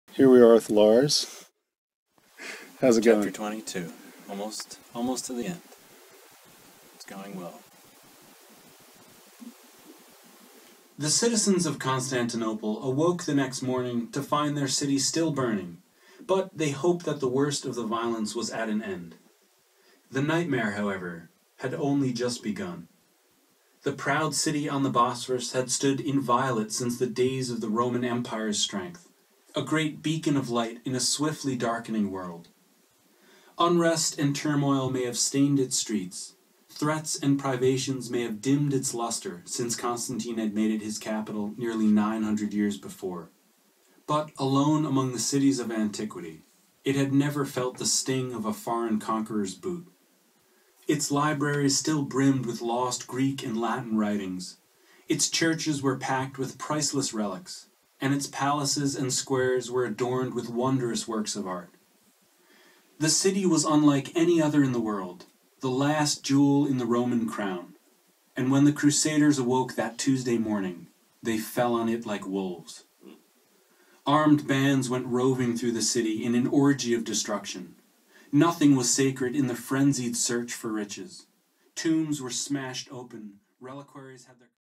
Recording the "Lost to the West" Audiobook
Lars Brownworth during the recording session of his book "Lost to the West: The Forgotten Byzantine Empire that Rescued Western Civilization". (apologies, I recorded this on my iPhone so the quality isn't ideal)